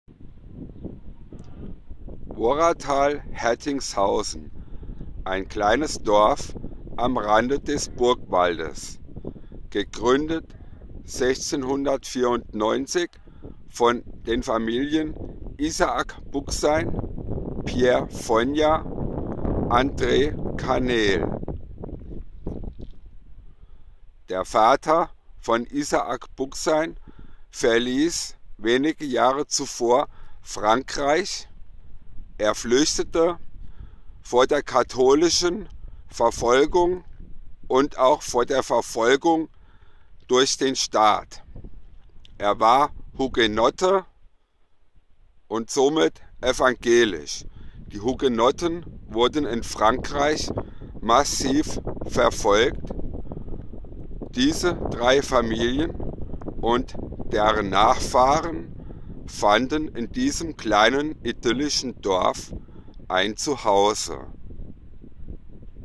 Hugenottendorf Hertingshausen am Burgwald
Dieses Video habe ich vor ein paar Jahren aufgenommen und ein paar Worte zur Gründung durch meine Vorfahren gesagt.
Es handelt sich nicht um ein professionelles Video. Es wurde mit einem Smartphone aufgenommen und ich bitte die mangelnde Qualität zu entschuldigen! Danke für`s Ansehen! :)